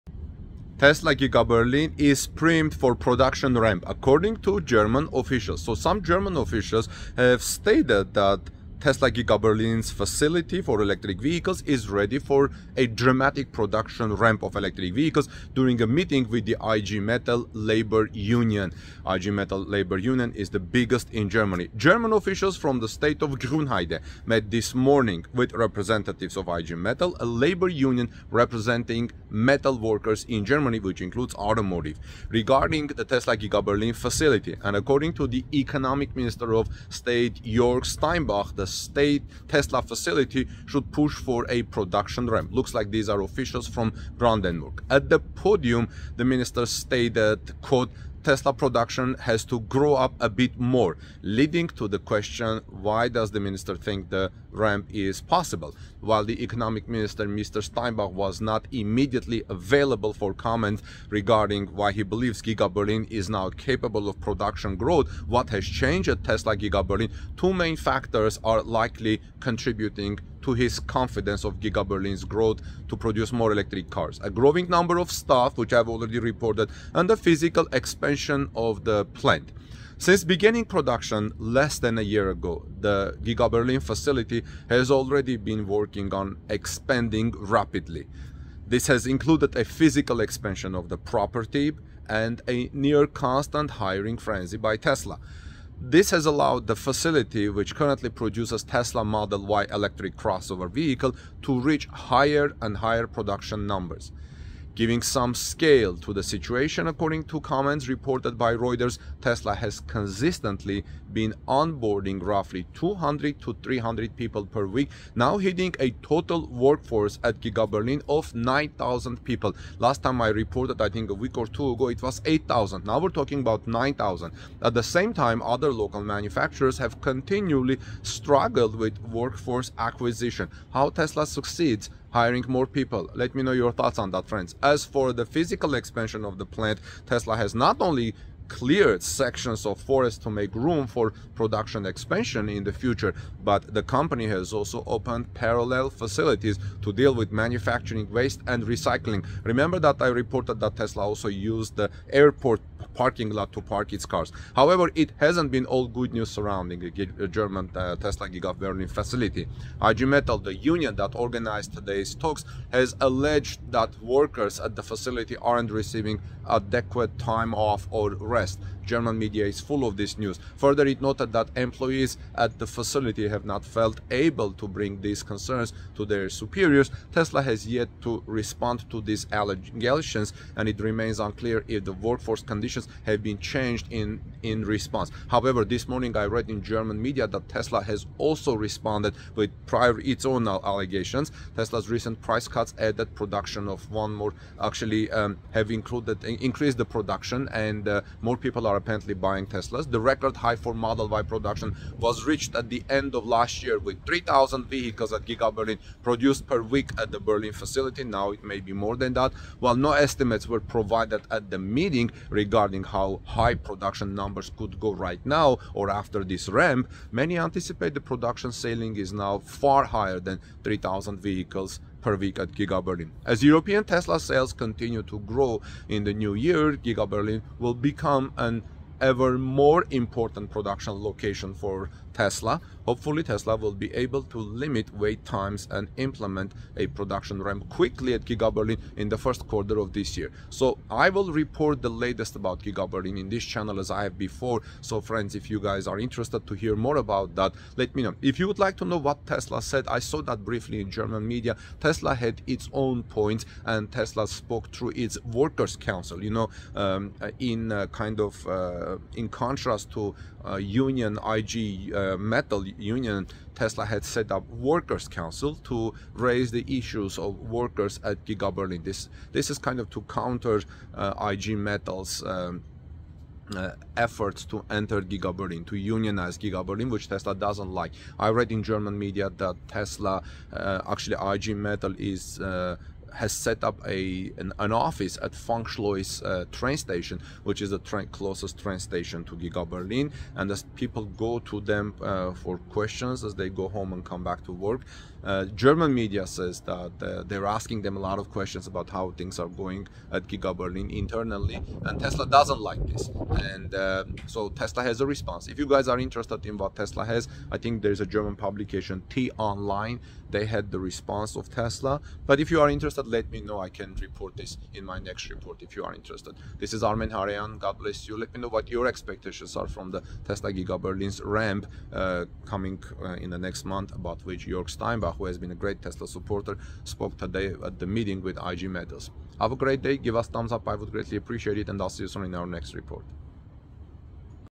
TESLA Giga Berlin Accelerating Towards the Finish Line With New Ramp
HOURS AGO: Tesla Giga Berlin is ready for a major production ramp.

German officials from the state of Brandenburg met this morning with representatives of IG Metall, a labor union representing metal workers in Germany, to discuss the Tesla Giga Berlin facility and its potential for a dramatic production ramp. The Economic Minister of the State, Jörg Steinbach, declared that the Tesla facility should push forward to achieve the desired production ramp. He further noted that all the necessary permits were obtained and the plant was ready to begin operations.

At the podium, the minister stated, “[Tesla production] has to grow up a bit more,” leading to the question, why does the minister think the ramp is possible?

The Tesla Giga Berlin facility is a major new manufacturing complex that is expected to create thousands of jobs in the area and bring economic growth to the region. The facility will also benefit from the local supply chain and workforce, making it an attractive hub for the electric car maker. The facility is set to produce Tesla's Model Y SUV, as well as the upcoming Cybertruck and other Tesla models.

The meeting between the officials and the representatives of the IG Metall was an important step towards the successful operations of the Tesla Giga Berlin facility. The labor union needs to ensure that the workers at the facility receive fair wages and that their rights are respected. With the support of the union, the Tesla facility should be able to ramp up production quickly and efficiently.